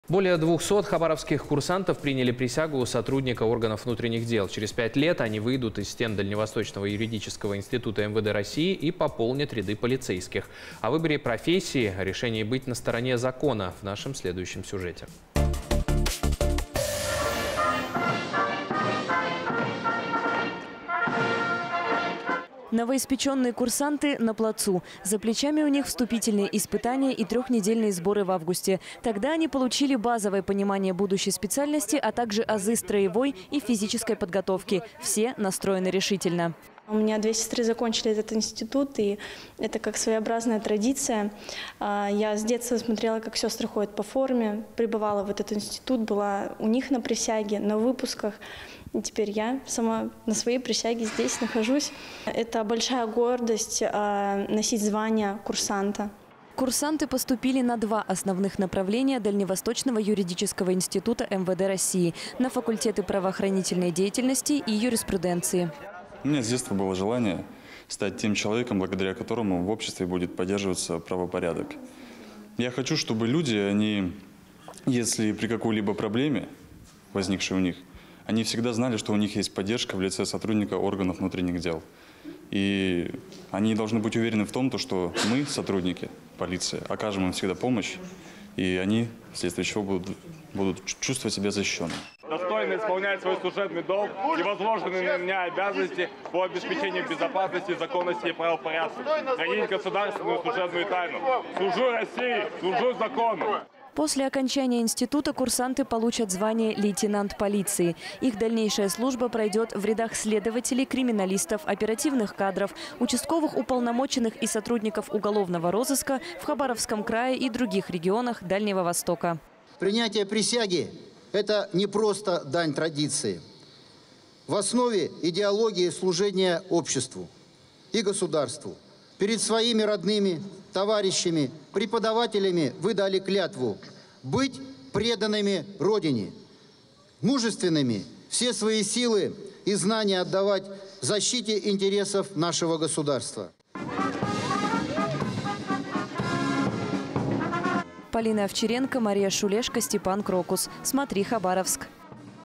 Курсанты Дальневосточного юридического института МВД РФ приняли присягу
Более 200 хабаровских курсантов приняли присягу сотрудника органов внутренних дел. Через пять лет они выйдут из стен Дальневосточного юридического института МВД России и пополнят
ряды полицейских.